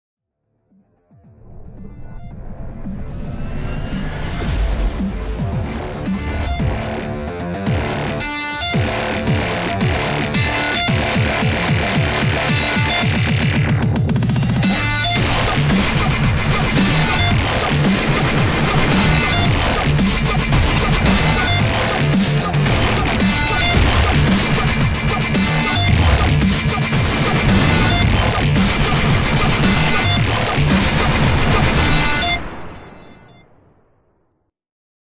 FIGHT | Mew2
A really sick edit from mew2, the flow and transitions are perfectly executed and the use effects used help empower the overall tone. This combines to create a fast paced and powerful edit. We hope you enjoy it as much as we did. Be sure to watch the video in full quality.
▬▬▬▬▬▬▬▬▬▬▬OPEN ME▬▬▬▬▬▬▬▬▬▬▬